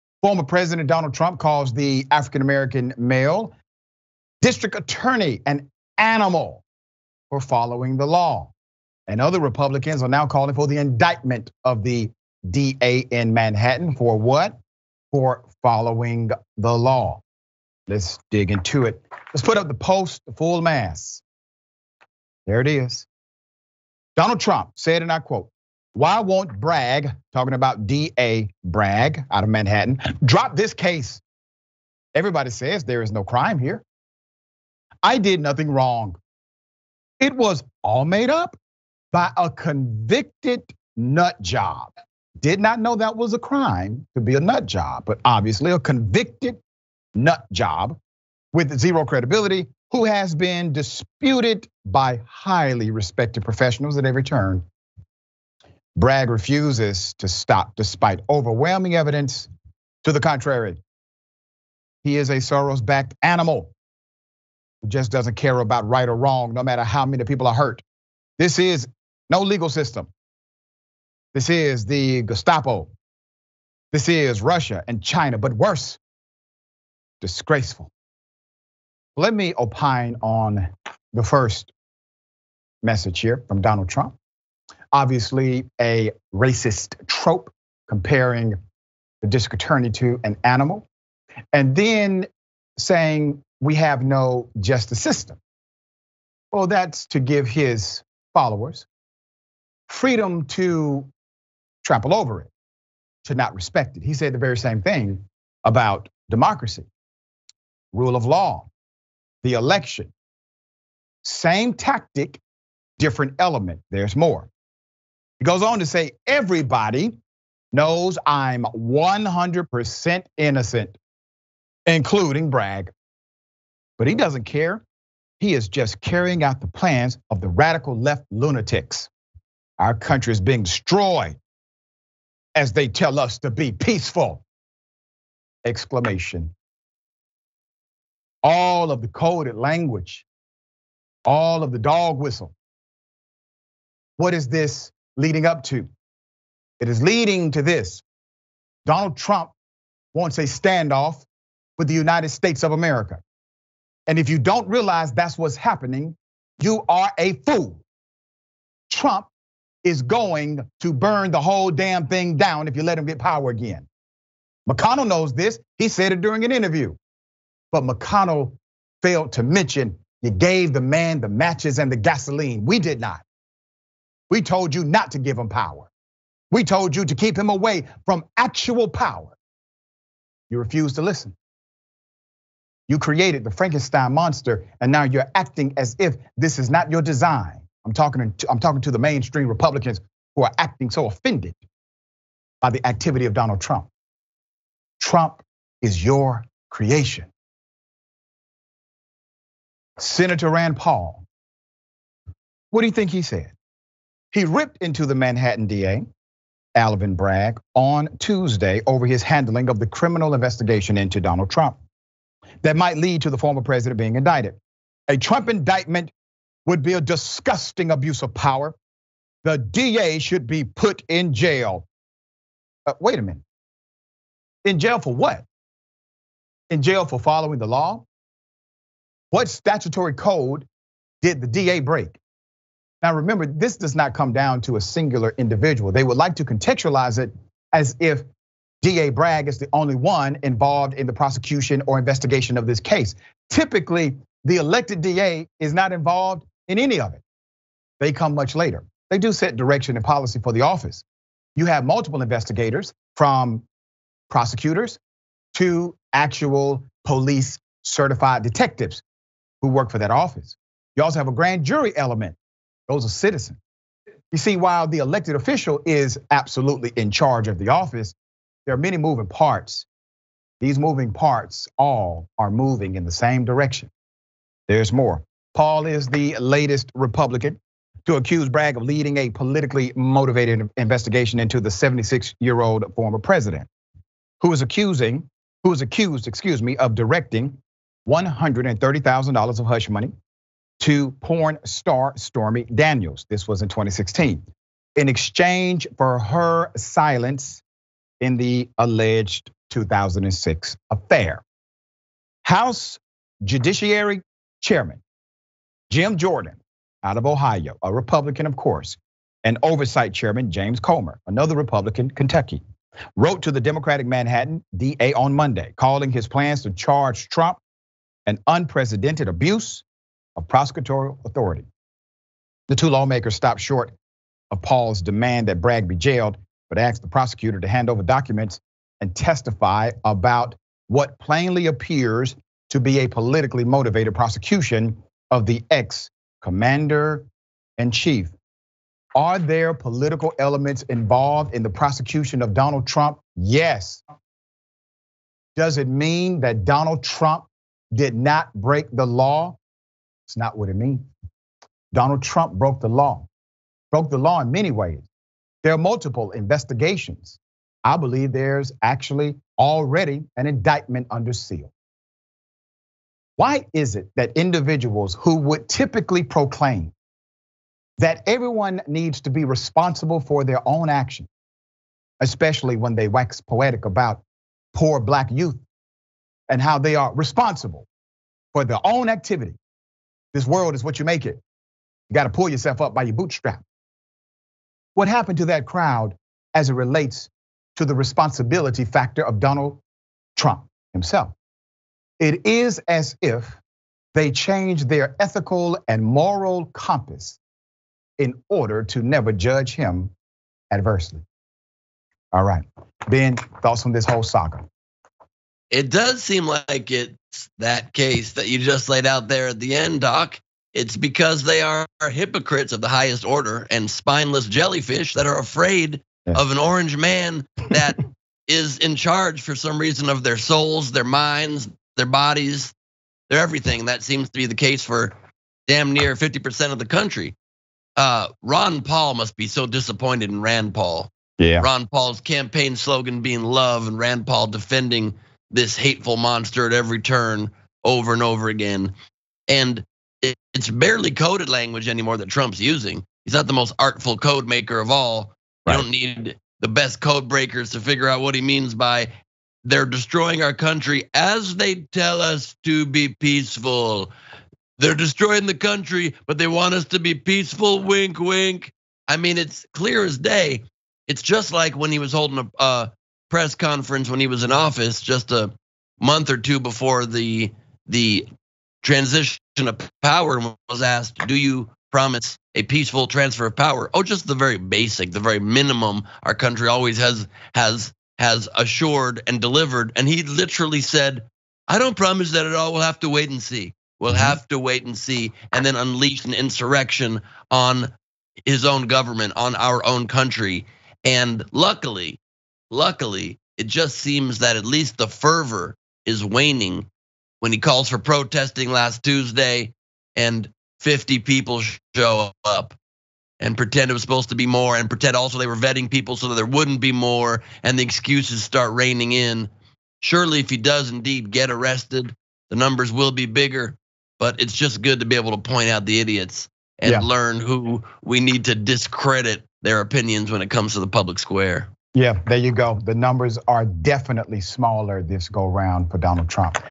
Trump Blows Racist Dog Whistle At Black Manhattan District Attorney
Donald Trump posted on Truth Social that Manhattan DA Alvin Bragg is a "Soros backed animal". Dr. Rashad Richey discusses on Indisputable.

"Donald Trump stepped up his personal attacks on Manhattan district attorney Alvin Bragg on Thursday as the wait goes on for the first prosecution of a former president in history. Trump doubled down on his criticism of his former lawyer Michael Cohen along with Bragg, who could indict Trump over alleged payments made to porn star Stormy Daniels to hush up an affair. “Why won’t Bragg drop this case?” Trump wrote on his Truth Social platform Thursday morning. “Everybody says there is no crime here. I did nothing wrong!” Apparently referring to Cohen, Trump continued: “It was all made up by a convicted nut job with zero credibility, who has been disputed by highly respected professionals at every turn. Bragg refuses to stop despite overwhelming evidence to the contrary. He is a Soros backed animal who just doesn’t care about right or wrong no matter how many people are hurt.” He added that there “is no legal system, this is the Gestapo, this is Russia and China, but worse. Disgraceful!” In a follow-up post, Trump claimed Bragg knows he’s innocent. “But he doesn’t care,” Trump added. “He is just carrying out the plans of the radical left lunatics. Our country is being destroyed, as they tell us to be peaceful!”"

230323__ID01Trump